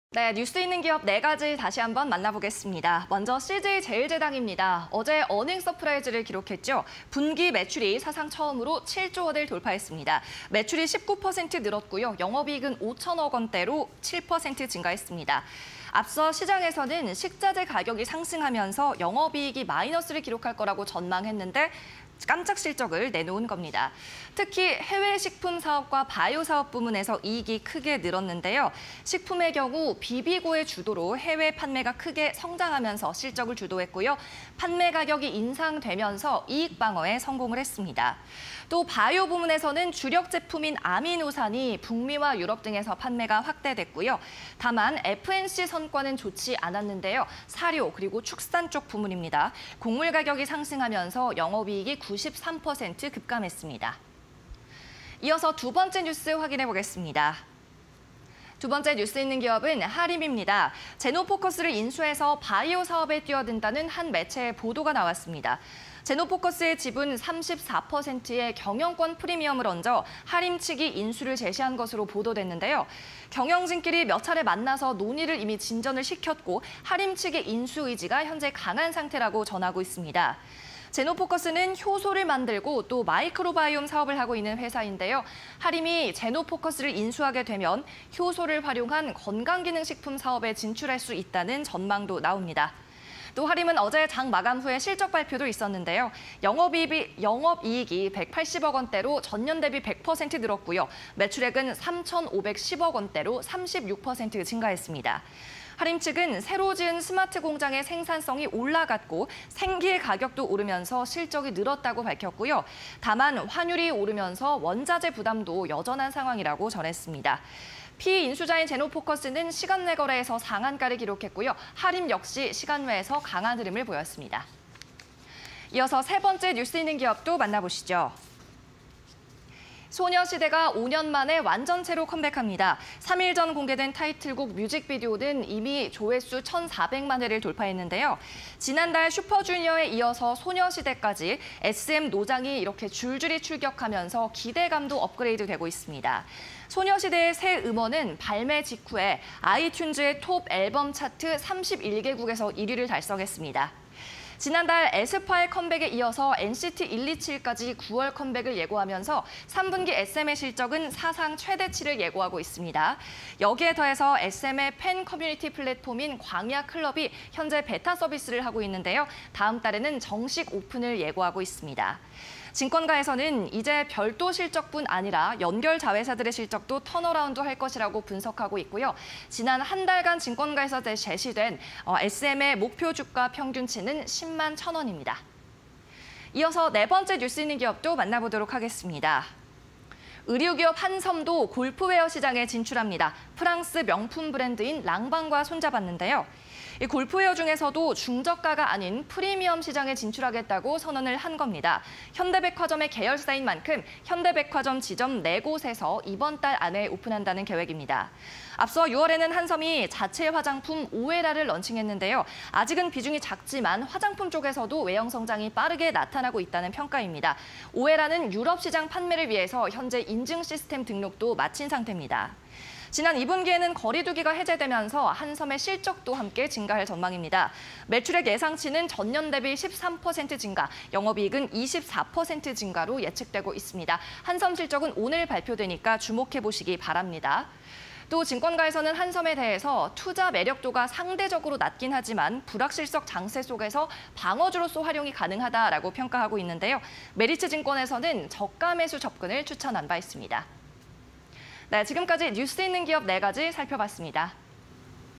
CJ제일제당, 하림, 에스엠, 한섬_공시&뉴스 (20220809)
- 매출 7조5166억·영업이익 5043억… ‘서프라이즈’
- 식품 부문 ▶비비고 중심 해외 판매 증가
- 바이오 부문 ▶아미노산 북미·유럽 판매 확대
- F&C 부문 ▶곡물가 상승으로 영업이익 급감

- “제노포커스 인수 타진” 보도… 바이오 뛰어드나
- 효소 활용해 건강기능식품 사업 진출 가능성
- 2분기 영업이익 180.5억… 전년 대비 100%↑
- 하림 측 “고환율로 원재료값 부담은 여전”

- 소녀시대 완전체 컴백… 신곡, 아이튠즈 31개국 1위
- 3분기 에스파·NCT127 컴백… 사상 최대 실적 예고
- 팬 플랫폼 ‘광야 클럽’ 9월 정식 오픈 예정
- 증권가 “연결 자회사 실적도 턴어라운드 할 것”

- 프리미엄 골프웨어 진출… ‘랑방블랑’ 론칭
- 佛 명품 브랜드 ‘랑방’과 손 잡아… 9월 오픈
- 오늘 실적 발표… 영업이익 24% 증가 추정
- 메리츠 “불확실성 속 방어주로 활용… 저가매수 접근”